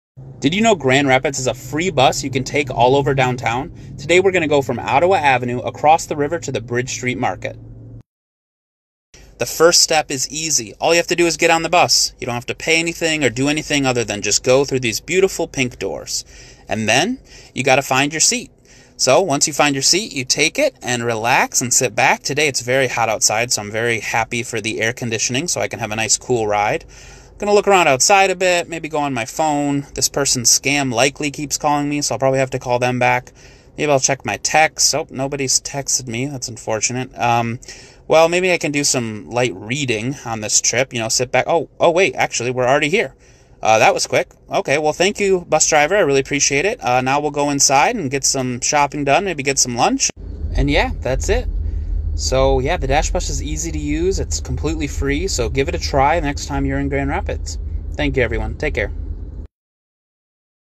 Did you know the City has a FREE-to-ride bus that goes all over downtown?
Grandrapidsmi.gov/dash

Check out the DASH bus next time you are downtown! It's easy to ride and completely free.

Filmed and edited by Anil!